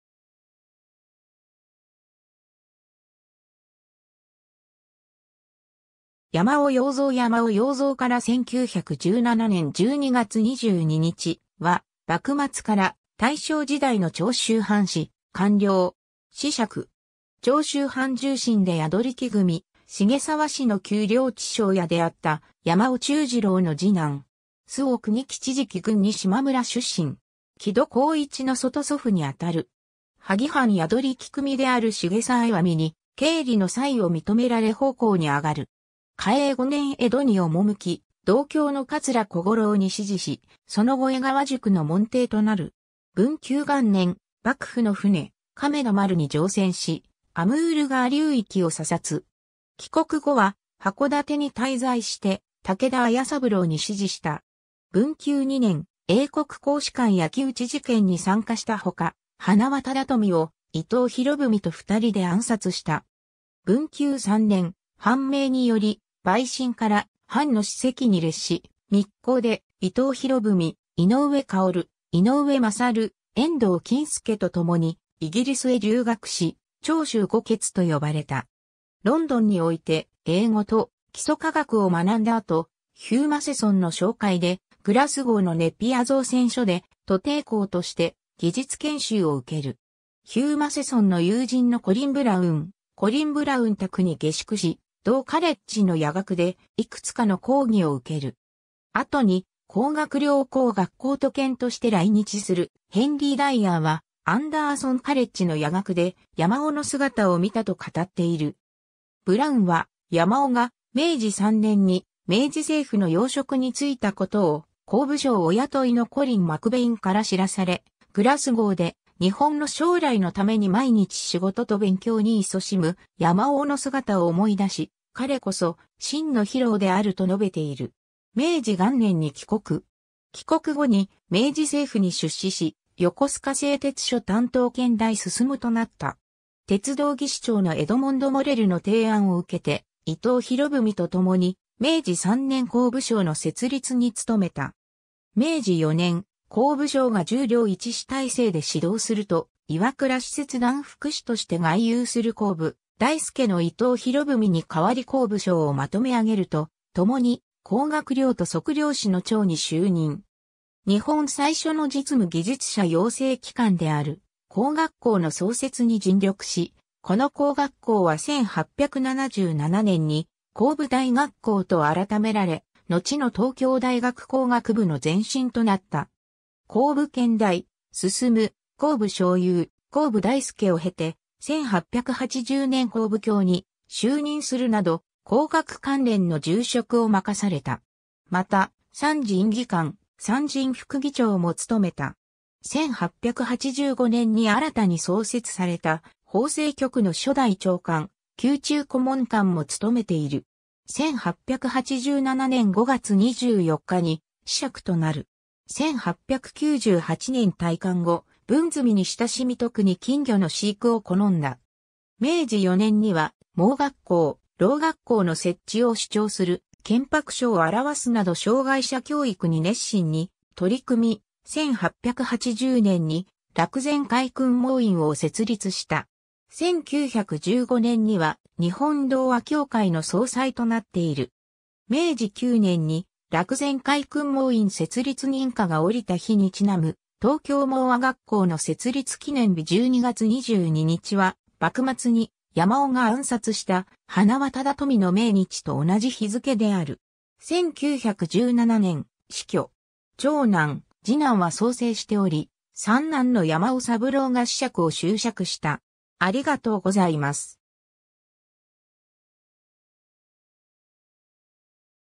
山尾庸三
造家学会・建築学会の人物
山尾庸三 山尾 庸三（やまお ようぞう、天保8年10月8日（1837年11月5日） - 1917年（大正6年）12月22日）は、幕末から大正時代の長州藩士、官僚。
子爵。
 長州藩重臣で寄組繁沢氏の給領地庄屋であった山尾忠治郎の二男。
周防国吉敷郡二島村（現・山口県山口市秋穂二島）出身。
木戸幸一（太平洋戦争時の昭和天皇側近）の外祖父（長女の寿栄が木戸の母）に当たる。
 萩藩寄組である繁沢石見に経理の才を認められ奉公（陪臣）に上がる。
嘉永5年（1852年）江戸に赴き、同郷の桂小五郎に師事し、その後江川塾の門弟となる。
 文久元年（1861年）、幕府の船・亀田丸に乗船し、アムール川流域を査察。
帰国後は箱館に滞在して武田斐三郎に師事した。
文久2年（1862年）、英国公使館焼き討ち事件に参加したほか、塙忠宝を伊藤博文とふたりで暗殺した。
 文久3年（1863年）、藩命により陪臣から藩の士籍に列し、密航で伊藤博文・井上馨・井上勝・遠藤謹助と共にイギリスへ留学し、長州五傑と呼ばれた。
 ロンドンにおいて英語と基礎科学を学んだ後、ヒュー・マセソン (Hugh Matheson) の紹介でグラスゴーのネピア造船所 (Napier Shipyard) で徒弟工として技術研修を受ける。
ヒュー・マセソンの友人のコリン・ブラウン Colin Brown宅に下宿し、同カレッジの夜学でいくつかの講義を受ける。
後に工学寮工学校都検として来日するヘンリー・ダイアーは、アンダーソン・カレッジの夜学で山尾の姿を見たと語っている。
 ブラウンは、山尾が明治3年に明治政府の要職に就いたことを工部省御雇いのコリン・マクヴェインから知らされ、グラスゴーで日本の将来のために毎日仕事と勉強に勤しむ山尾の姿を思い出し、彼こそ「真のヒロー」であると述べている。
 明治元年（1868年）に帰国。
帰国後に明治政府に出仕し、横須賀製鉄所担当権大丞となった。
鉄道技師長のエドモンド・モレルの提案を受けて伊藤博文とともに明治3年（1870年）工部省の設立に勤めた。
 明治4年（1871年）、工部省が10寮1司体制で始動すると、岩倉使節団副使として外遊する工部大輔の伊藤博文に代わり工部省をまとめあげるとともに、工学寮と測量司の長に就任。
日本最初の実務技術者養成機関である工学校の創設に尽力し、この工学校は1877年に工部大学校と改められ、のちの東京大学工学部の前身となった。
工部権大丞・工部少輔、工部大輔を経て、1880年工部卿に就任するなど、工学関連の重職を任された。
 また、参事院議官、参事院副議長も務めた。
1885年に新たに創設された法制局の初代長官、宮中顧問官も務めている。
1887年5月24日に子爵となる。
 1898年退官後、文墨に親しみ特に金魚の飼育を好んだ。
 明治4年には盲学校、聾学校の設置を主張する建白書を表すなど障害者教育に熱心に取り組み、1880年に楽善会訓盲院(東京盲唖学校の前身)を設立した。
1915年には日本聾唖協会の総裁となっている。
明治9年(1876年)に楽善会訓盲院設立認可が下りた日に因む東京盲唖学校の設立記念日12月22日(新暦)は、幕末に山尾が暗殺した塙忠宝(盲目の国学者塙保己一の息子)の命日と同じ日付(旧暦)である。
 1917年、死去。
 長男・次男は早世しており、三男の山尾三郎（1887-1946年）が子爵を襲爵した。